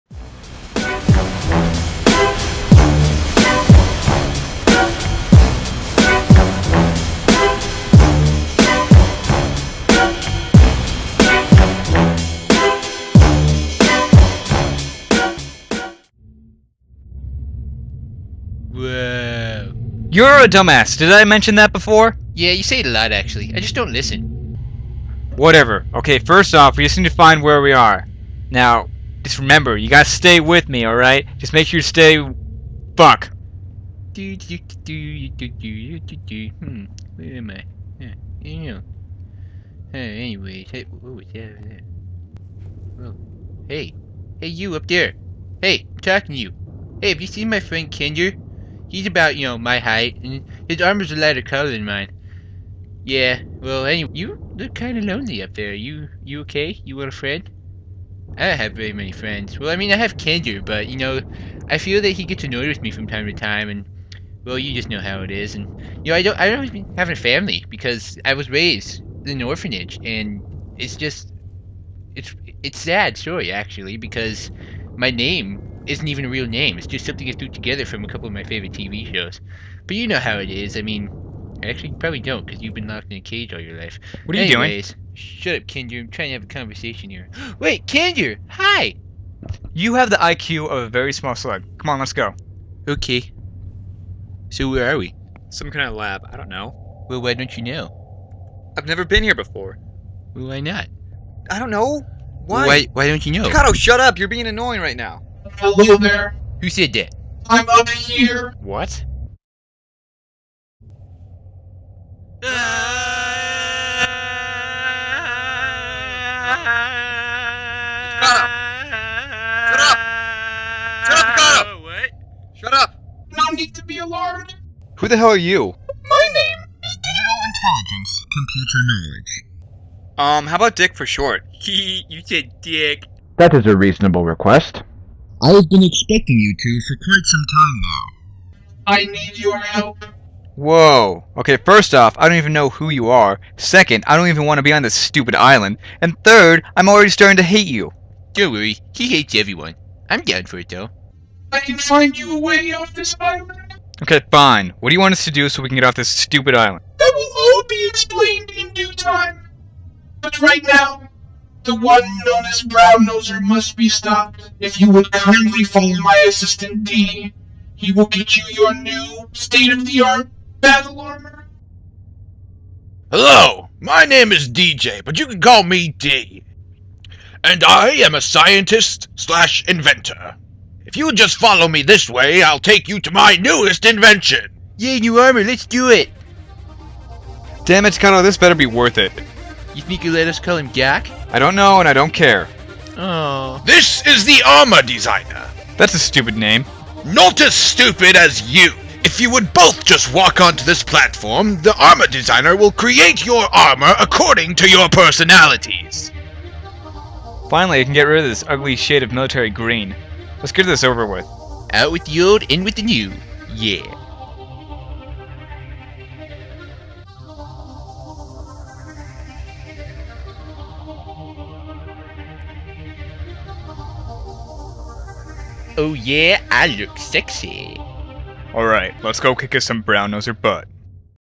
Dropouts Episode 3 Fun with Dick and Dee
our heroes find themselves in the lab of the Islands super computer where they decide to help Dick and they get new armor.